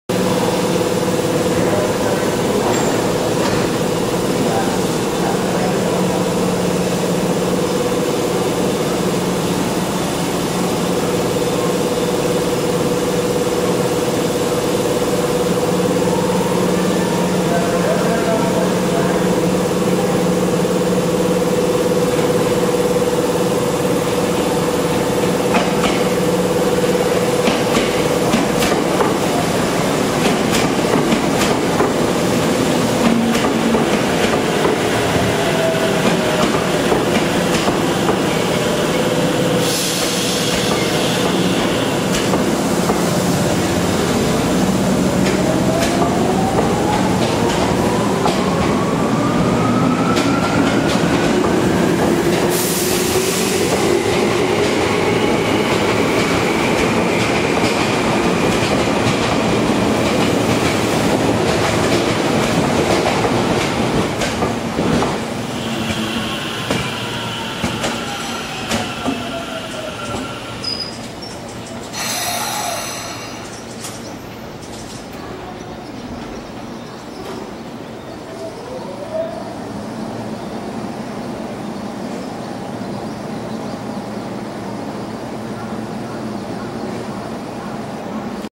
東京地下鉄08系（準急長津田行）東武動物公園に入線
東武伊勢崎線東武動物公園に入線する、東京メトロ08系。当駅始発半蔵門線・東急田園都市線直通準急長津田行。東武線内と東急線内は準急。半蔵門線内は各停である。